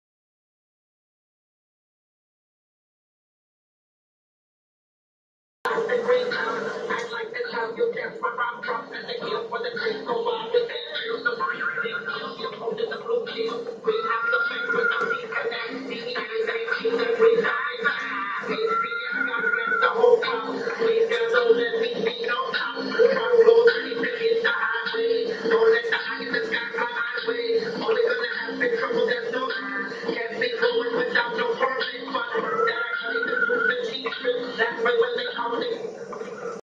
EHW Arrivals! Dr. Greenthumb
EHW Arrivals...the ehw international champion Dr greenthumb explodes into the ring at ehw carnage June 2000